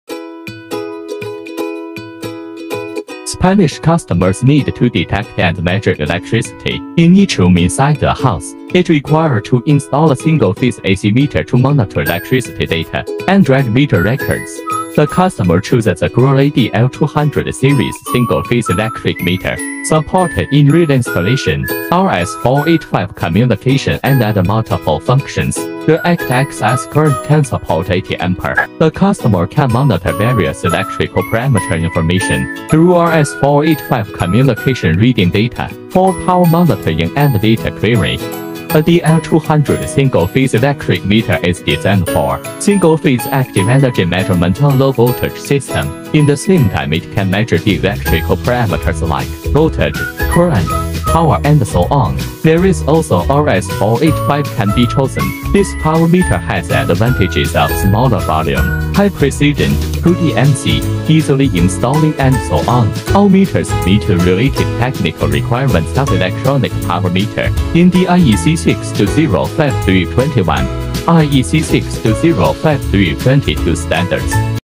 Acrel Electric | ADL200 Single Phase kWh Meter Applied in Spanish Residential Electricity Metering
ADL200 single phase din rail electric kWh energy meter is designed for single phase active energy measurement on low voltage system, in the same time it can measure the electrical parameters like voltage, current, power and so on. There is also RS485 communication can be chosen.

LCD display and electricity pulse output function is installed in the power meter; clock, rate period parameters can be set also. Data swap with the super ordination machine via RS485 communication interface greatly facilitate the power automated administration. With MID certified.